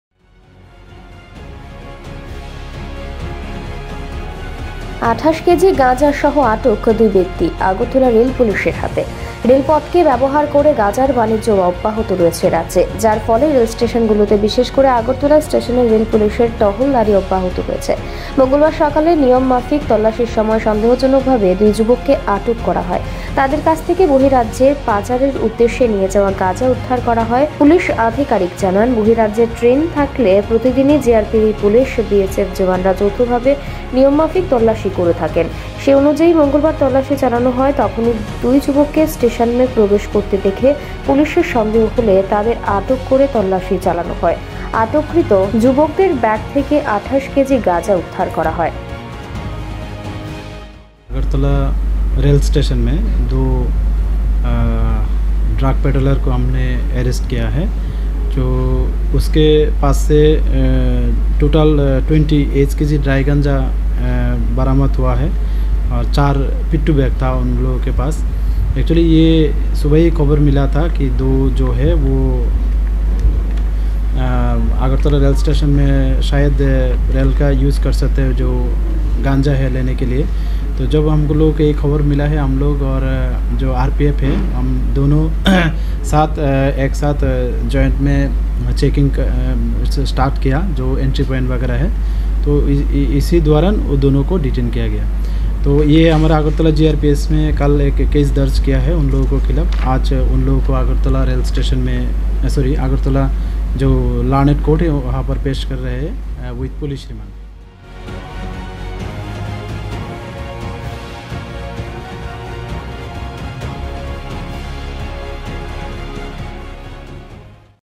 Agartala : ২৮ কেজি গাজাঁ সহ আটক ২ ব্যক্তি আগরতলা রেল পুলিশ এর হাতে || VOICE 18
VOICE 18
Name of Production House: FLAXEN MEDIA WORKS